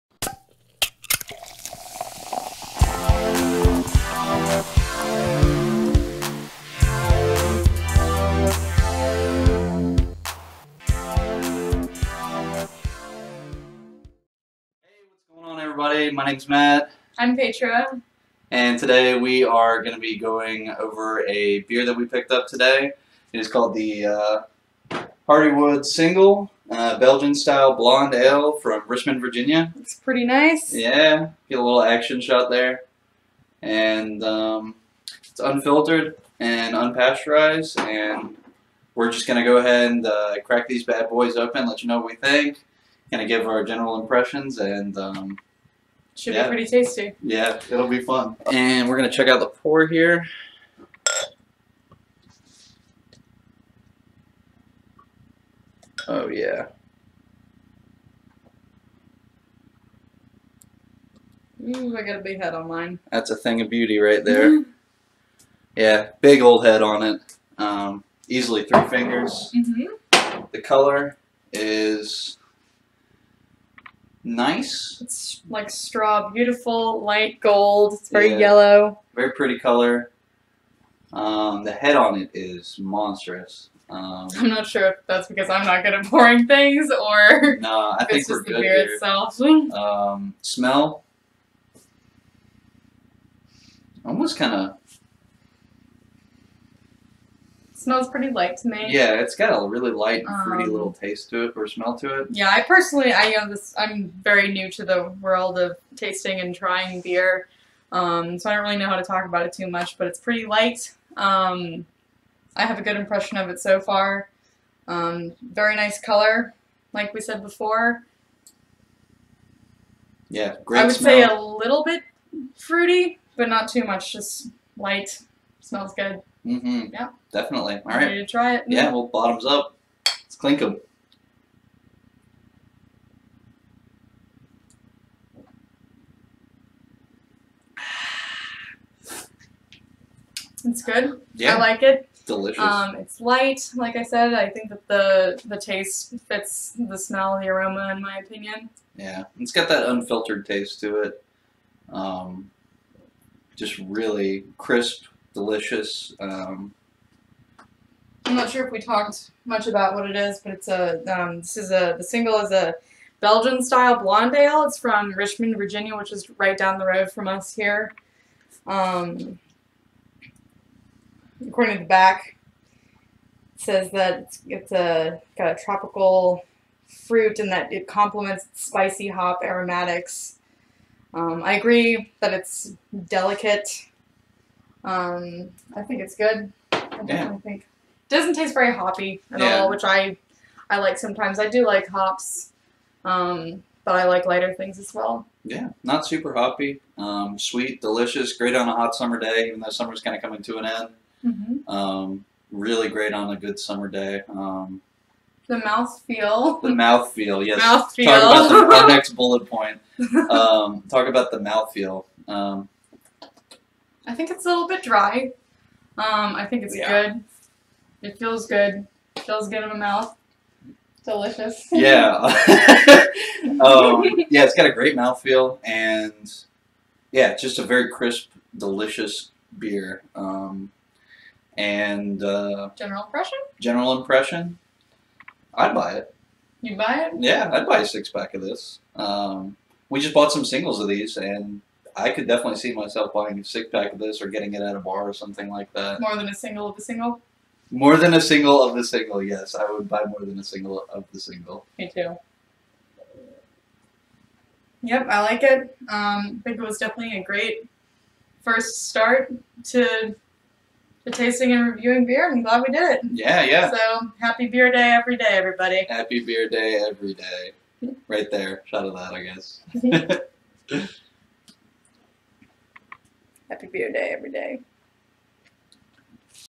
Hardywood Singel Under The Cap Beer Review - Hop N' Slop
We review this tasty brew from Hardywood down in good ole Richmond, VA.